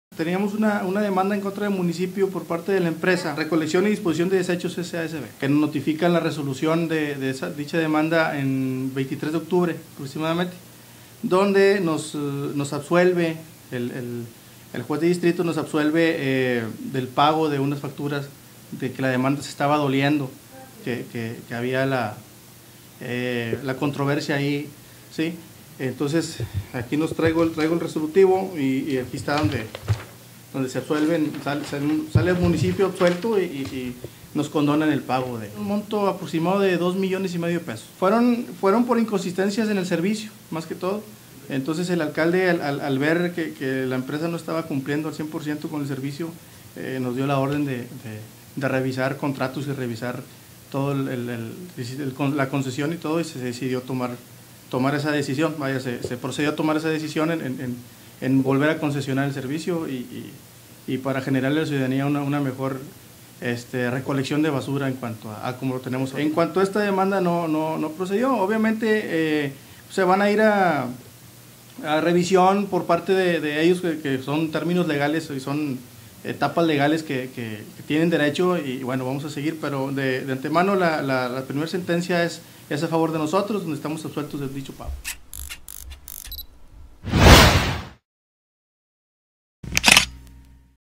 GANA MUNICIPIO DE ALLENDE N.L DEMANDA MILLONARIA.
Allende.-Un Juez de distrito notifica al municipio que resulta absuelto de la demanda en contra del Municipio la cual fue interpuesta por parte de la empresa Recolección y Disposición de Deshechos S.A de C.V. de 2.5 millones de pesos.